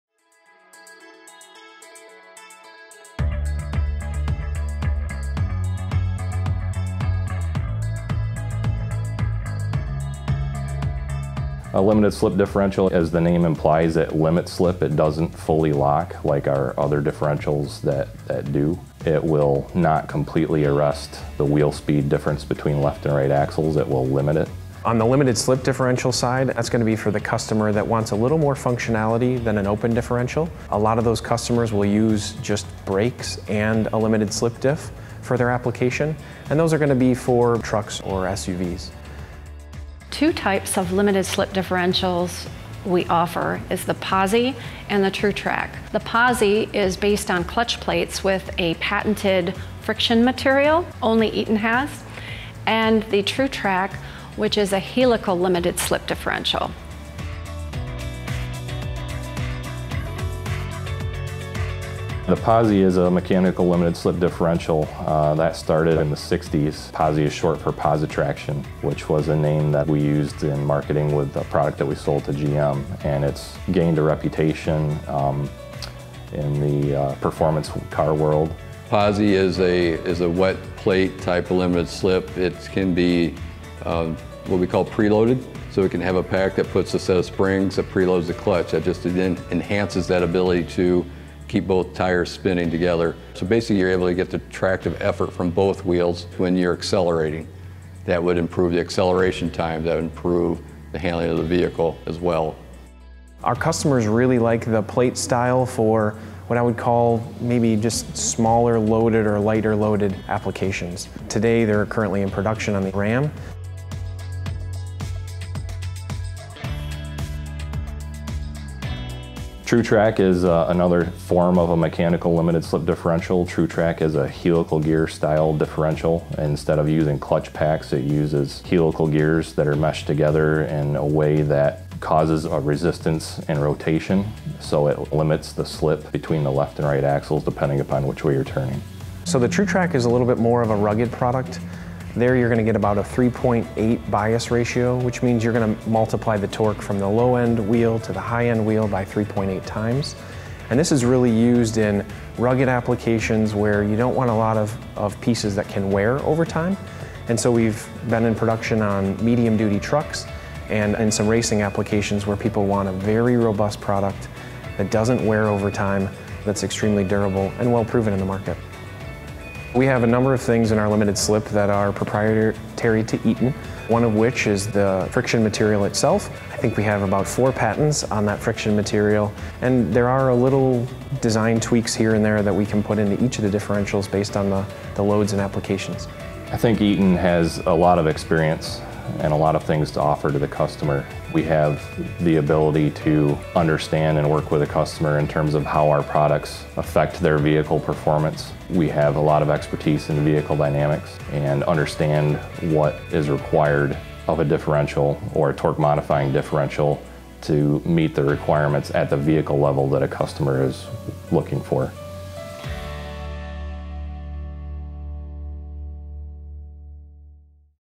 Eaton Limited Slip Differentials - portfolio and capabilities
Eaton experts review our Limited Slip differential portfolio and capabilities.